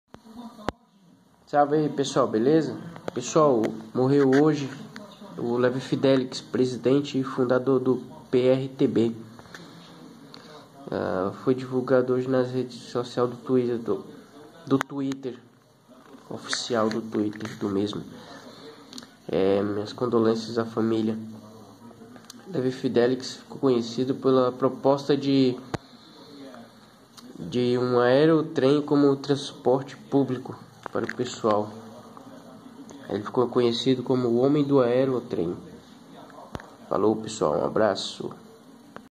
Levi Fidelix
morre o fundador do PRTB Levo Fidelix.